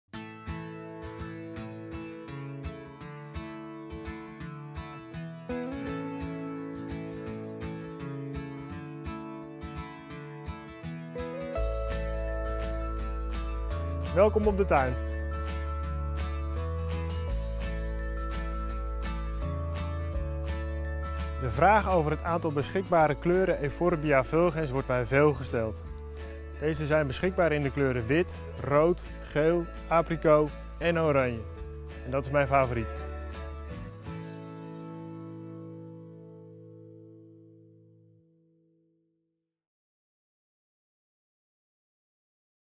Euphorbia fulgens; the grower invites you into his glass house
A quick look in the glass house of Euphorbia fulgens grower Pieter Beerepoot. Perfect autumn colours; do you have a favorite?